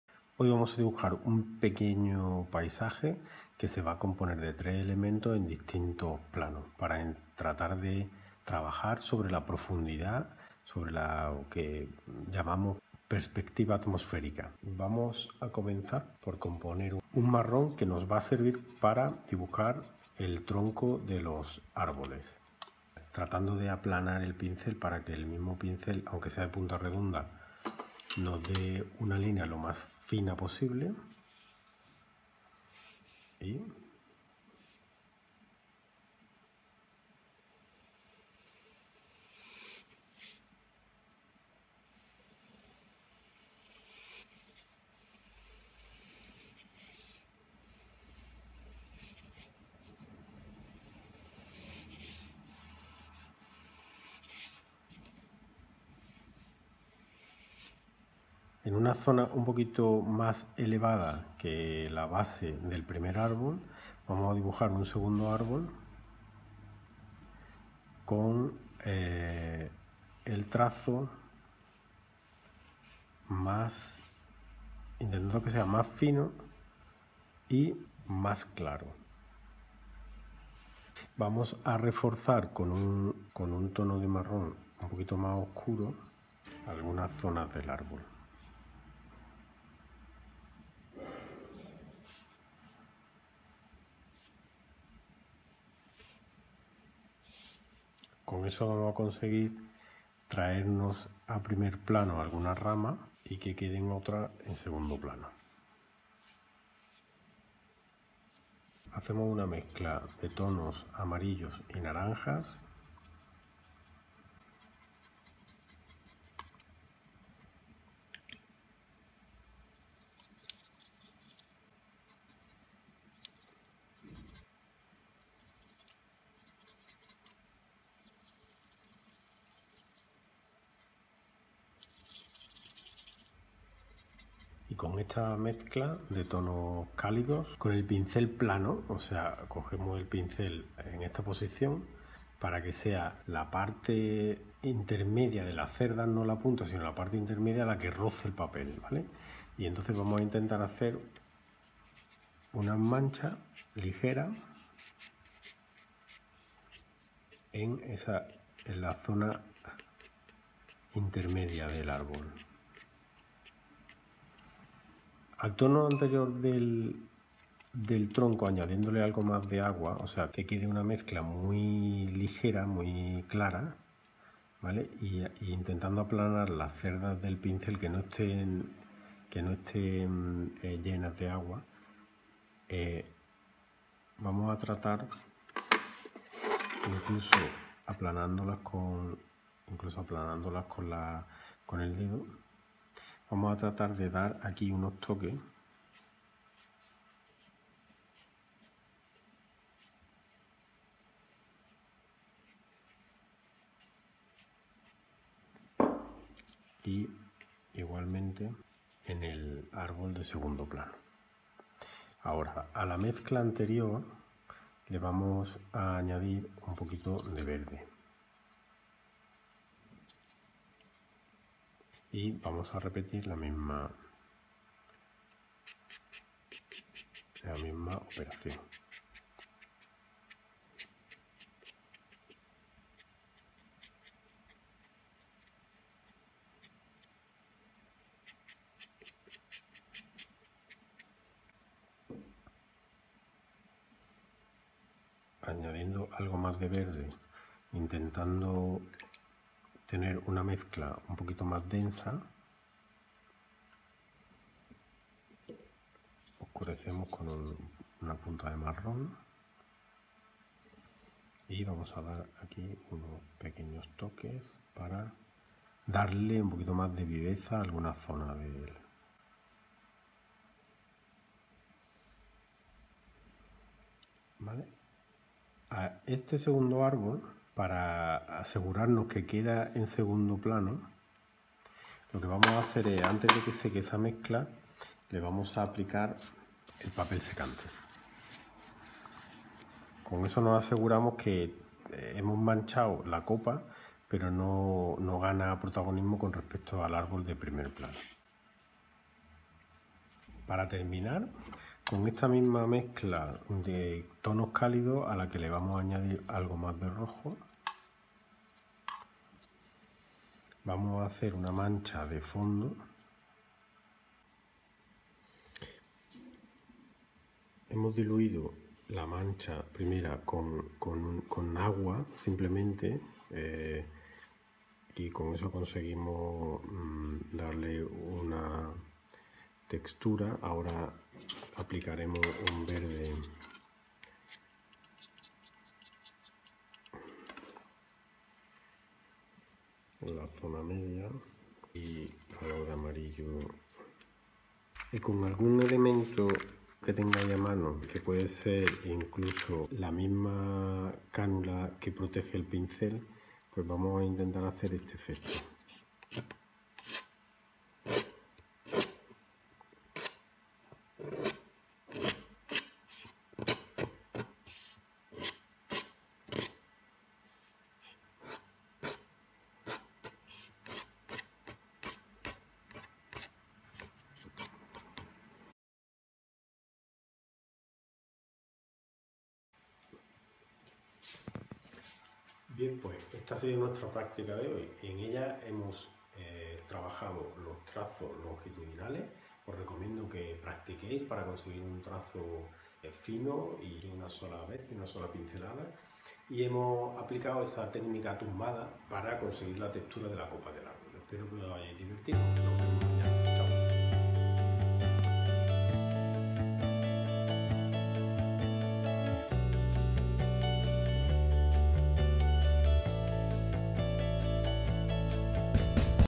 04 Dibujar con acuarela un mini paisaje con árboles
En esta práctica vamos a dibujar tres elementos en distintos planos, intentando componer un pequeño paisaje con profundidad.